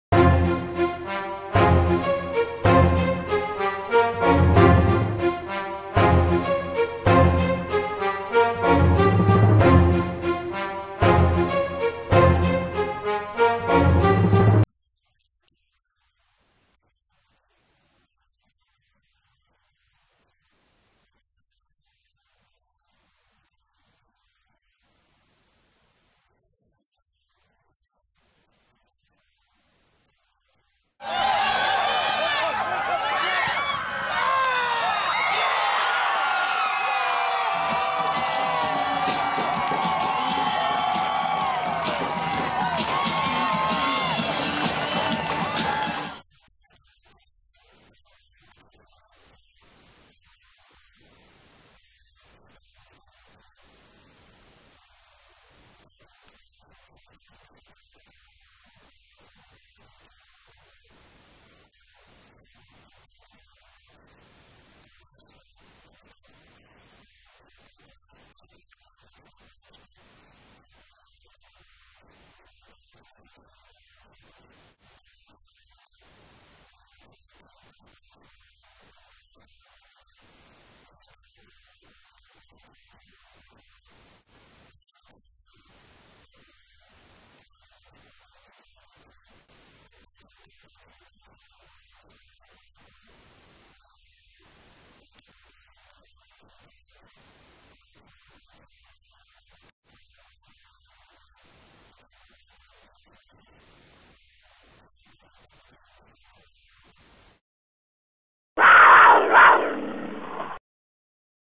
Binghamton University Men's Soccer Interviews/Highlights vs. Colgate (10/18/2011)
The Binghamton men's soccer team tied Colgate 1-1 Tuesday evening at the Bearcats Sports Complex. Sophomore forward Luke Halberg discusses his goal in the 58th minute to tie the match. Head coach Paul Marco talks about the performance of his team following their second straight tie.